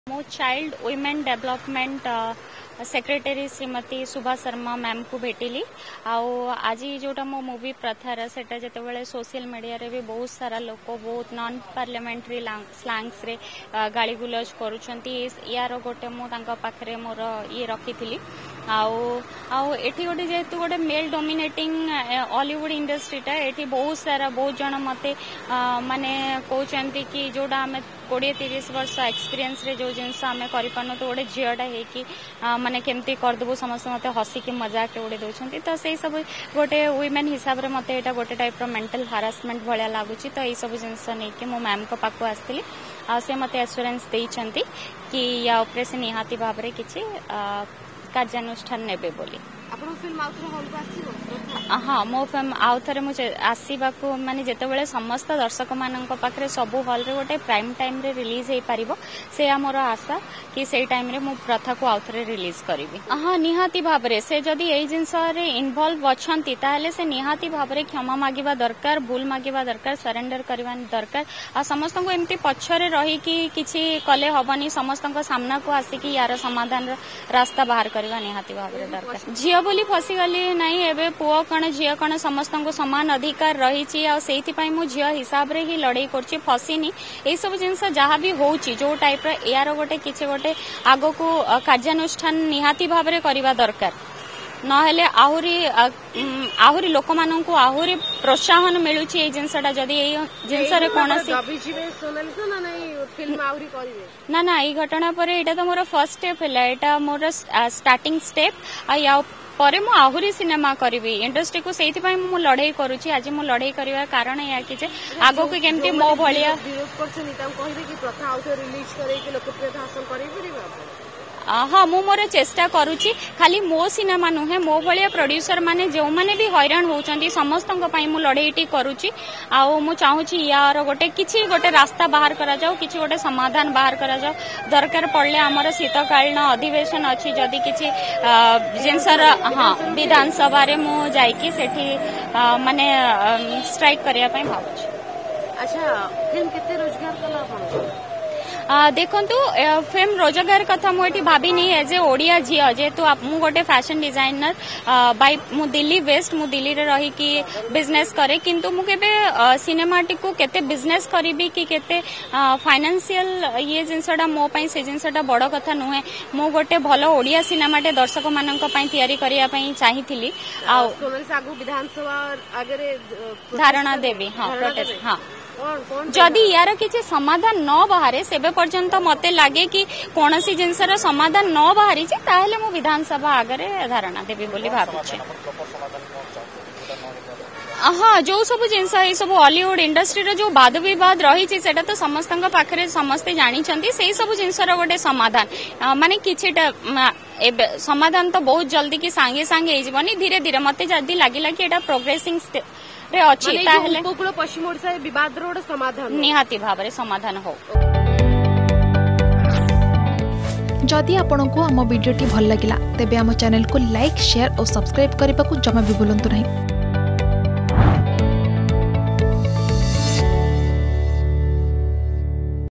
ସୋସିଆଲ ମିଡିଆରେ ପ୍ରଥା ସିନେମା ପ୍ରଯୋଜକ ସନଲିସାଙ୍କୁ ଟାର୍ଗେଟ || Sanlisa Patel
ସୋସିଆଲ ମିଡିଆରେ ପ୍ରଥା ସିନେମା ପ୍ରଯୋଜକ ସନଲିସାଙ୍କୁ ଟାର୍ଗେଟ । ଅସଭ୍ୟ ଭାଷାରେ ଗାଳି ଗୁଲଜ କରିବା ସହ ମଜା ଉଡାଇଲେ ଭ୍ୟୁୟର୍ସ ।  ଯାହାକୁ ନେଇ ମହିଳା ଓ ଶିଶୁ ବିକାଶ, ମିଶନଶକ୍ତି ସଚିବ ଶୁଭା ଶର୍ମାଙ୍କୁ ଭେଟିଛନ୍ତି ସନଲିସା ।